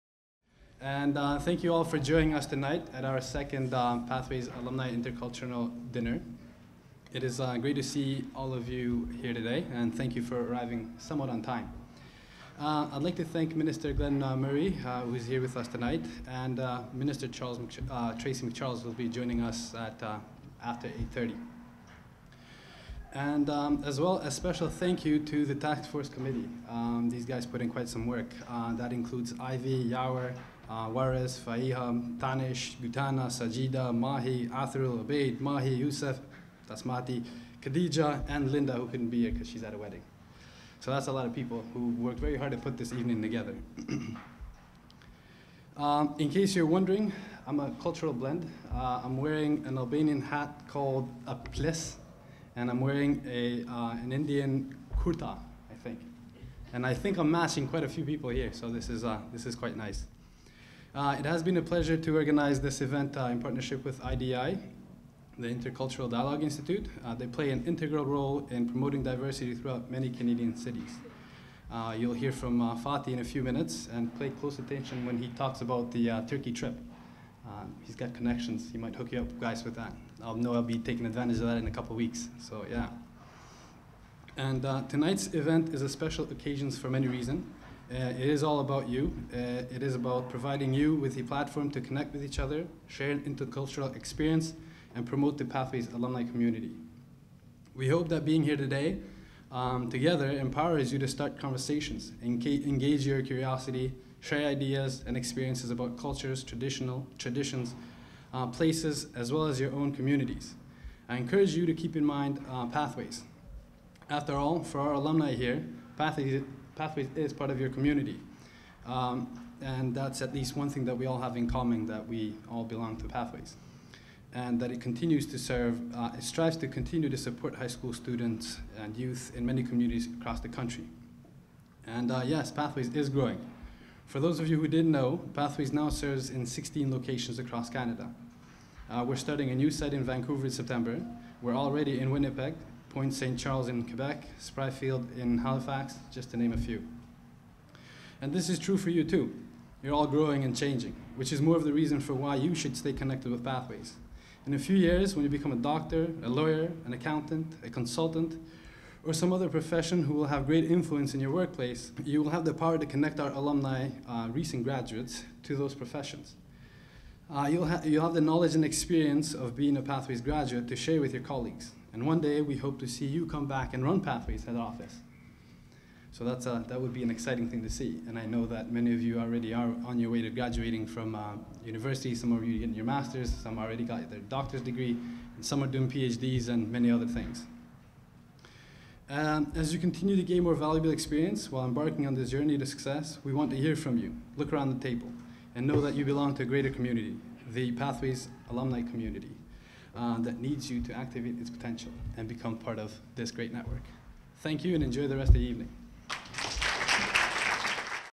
Selim Berbatovci, Pathways Canada
IDI GTA and Pathways to Education Canada collaborated to host the second Pathways Alumni Intercultural Dinner. The event provided a friendly platform to celebrate diversity, engage in dialogue, and enjoy an intercultural experience.  Pathways graduates and guests had the pleasure of hearing from distinguished guests Glen Murray, who shared his thoughts about our multicultural society and his personal experience leading in a diverse society, and Tracy MacCharles who talked about strengthening our communities through continued learning, giving back, and diversity.  Additionally, the event featured live Ebru Art performed by Serap Bulsen, and shared stories from Pathways graduates.